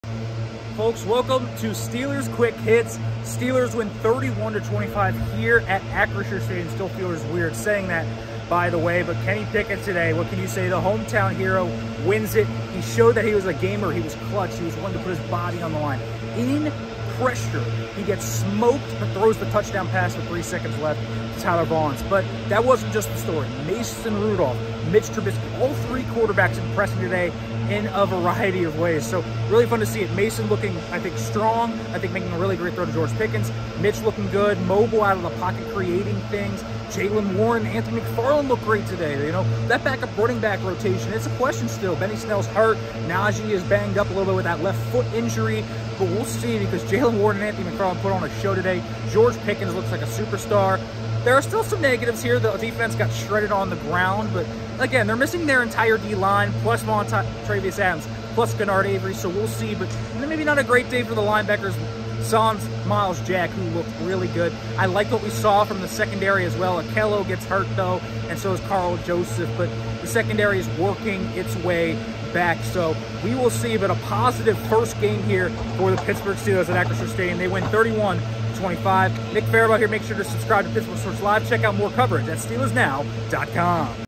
Steelers Postgame Quick Hits: Kenny Pickett, Mitch Trubisky, Mason Rudolph Shine in Steelers Win
Nick Farabaugh discusses the Steelers' 32-25 victory over the Seattle Seahawks at Acrisure Stadium, including the impressive play from all three quarterback, George Pickens doing what he does, and more.